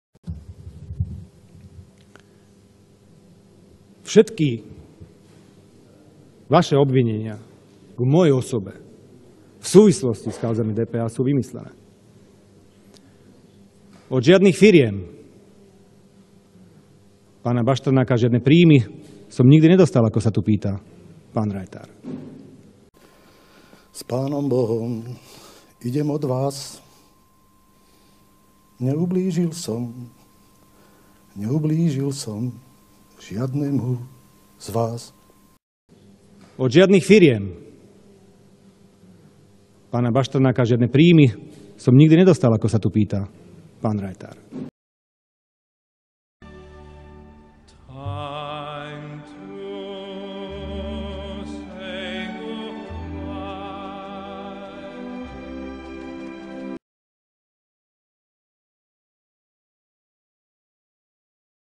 Dostal či nedostal Kaliňák príjmy od Bašternáka?
"Všetky vaše obvinenia k mojej osobe v súvislosti s kauzami DPH, sú vymyslené. Od žiadnych firiem pána Bašternáka som žiadne príjmy nikdy nedostal," uviedol minister Kaliňák v rozprave v parlamente. Viac o kauze